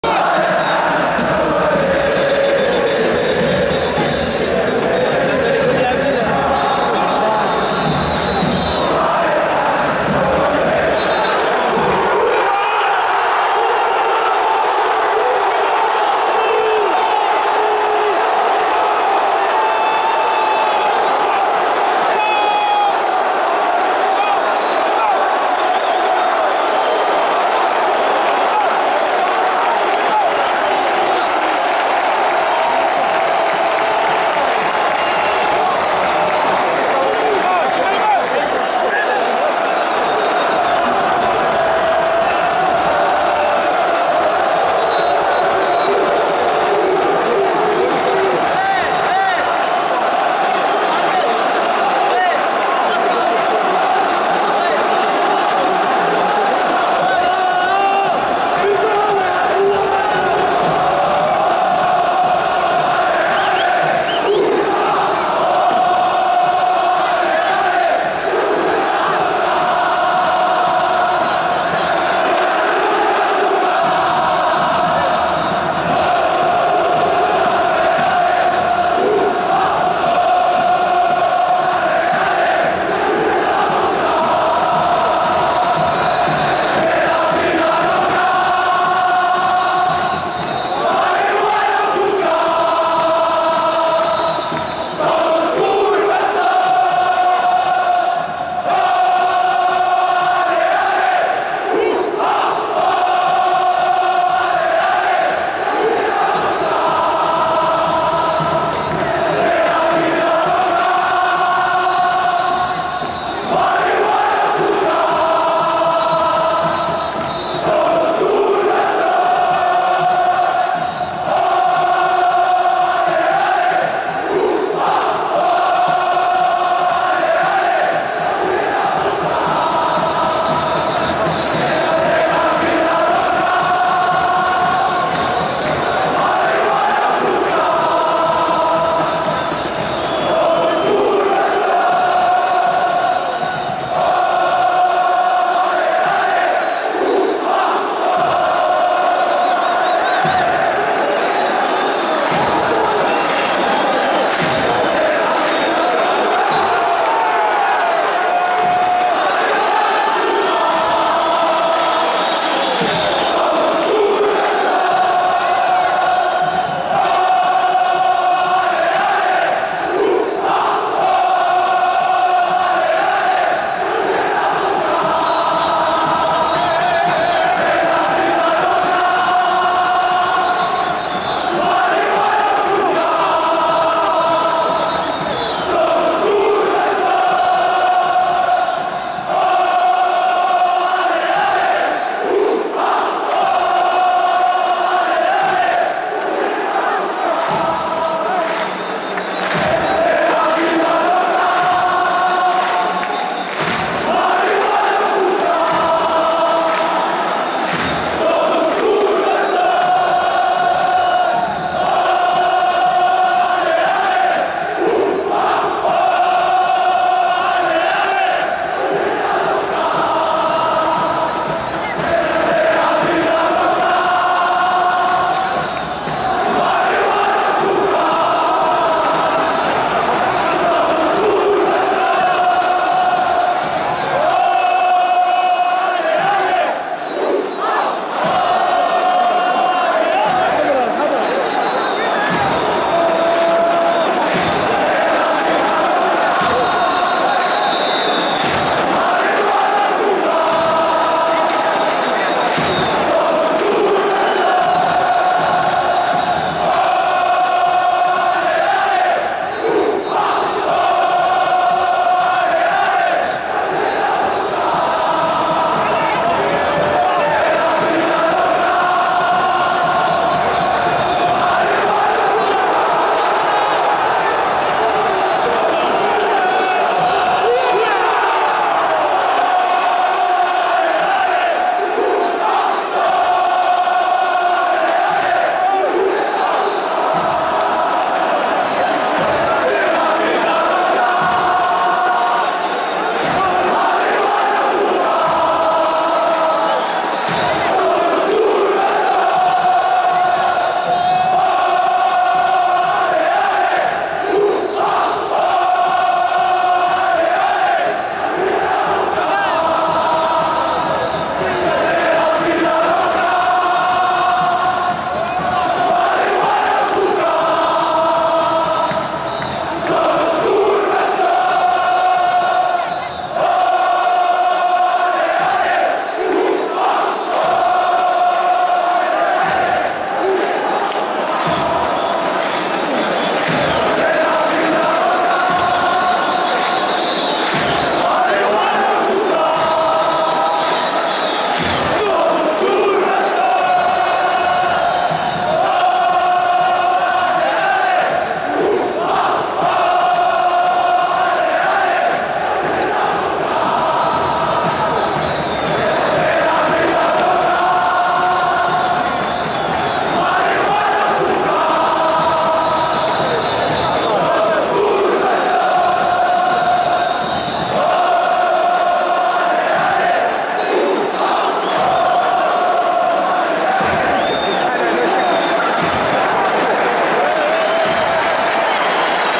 مباشرة من المدرجات هدف اللاعب زكرياء الهبطي للرجاء في مرمى الجيش الملكي وردة الفعل الهستيرية للجمهور
Raja Casablanca vs Far  1 - 0  pour la 14 journée de championnat du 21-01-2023 , مباشرة من المدرجات هدف اللاعب زكرياء الهبطي للرجاء في مرمى الجيش الملكي وردة الفعل الهستيرية للجمهور
SOMOS CURVA SUD
Curva Sud Casablanca
Page Officielle :
Chaine Zemama35/youtube